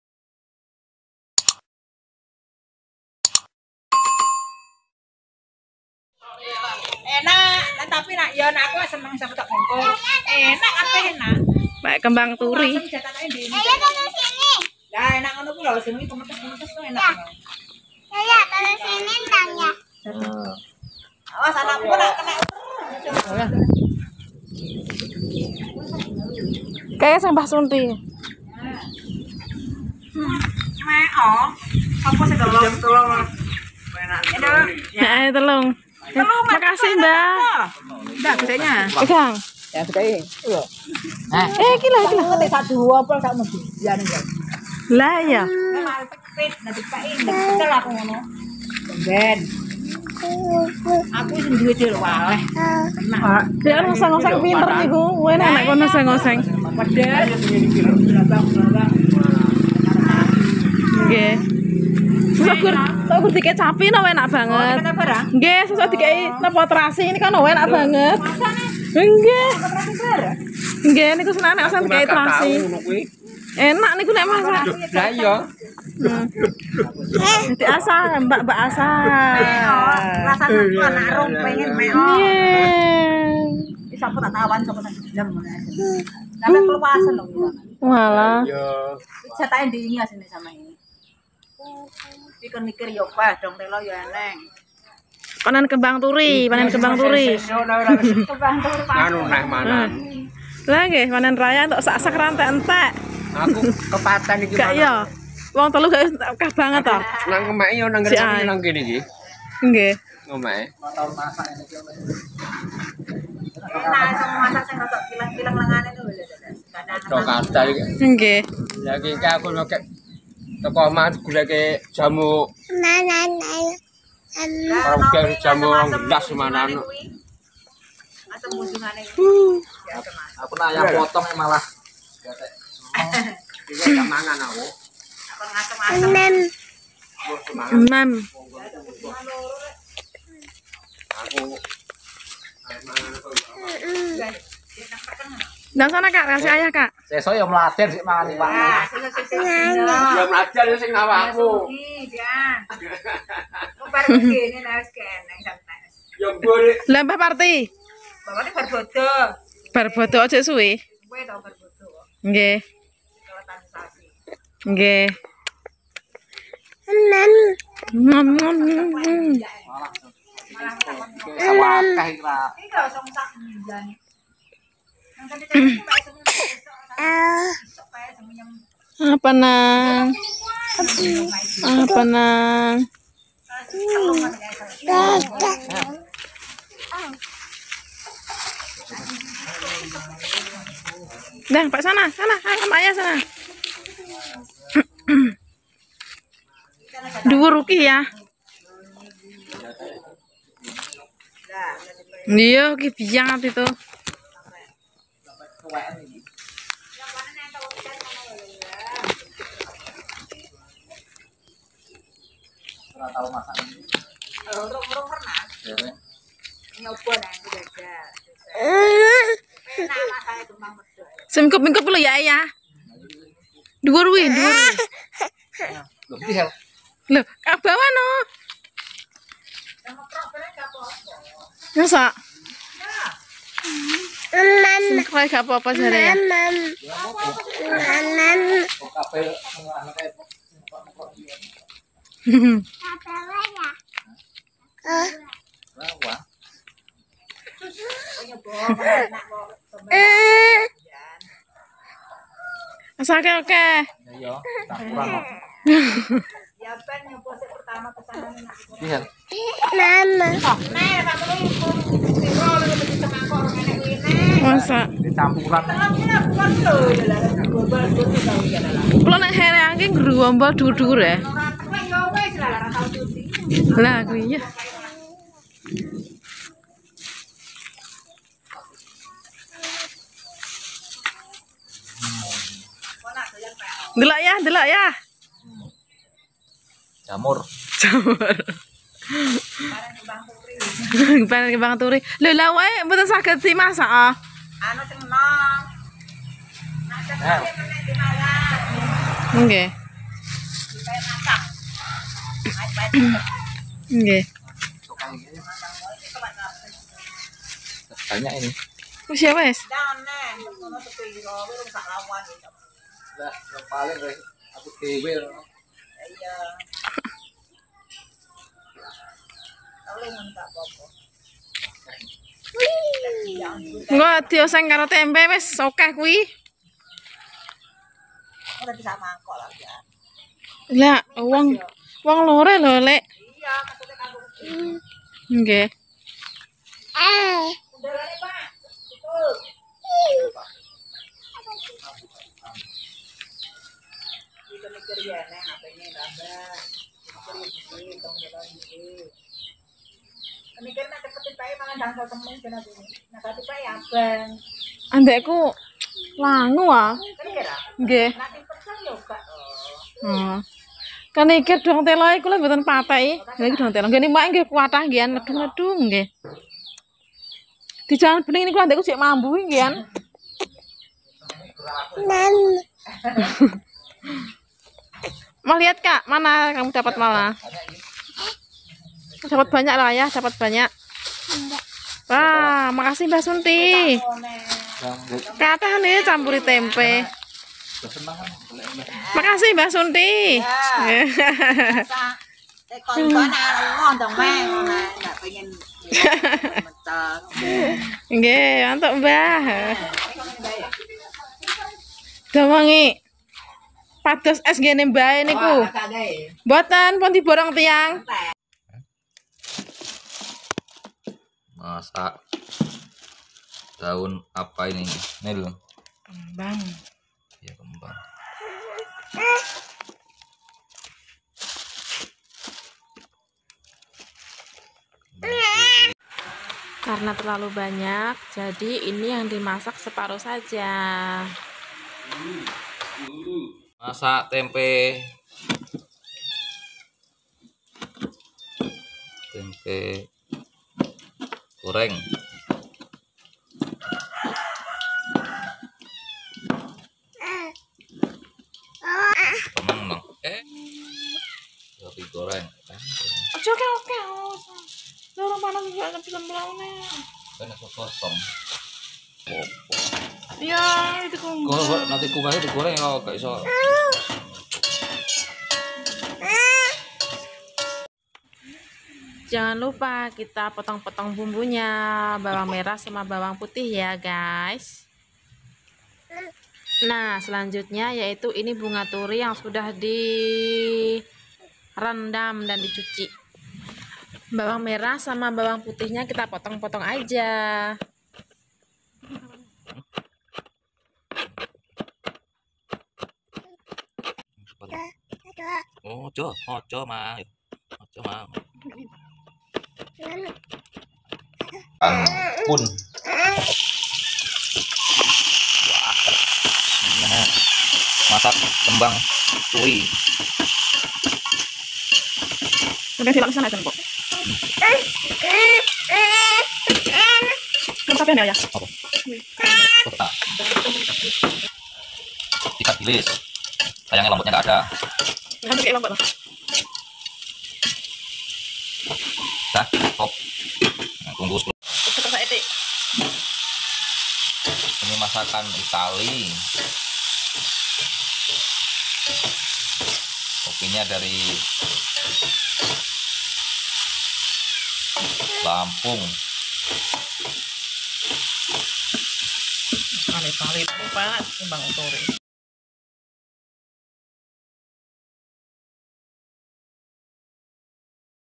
Cara Memasak Bunga Turi Tidak Pahit Di Jamin Semua Suka
Ide kreatif memasak bunga Turi tidak pahit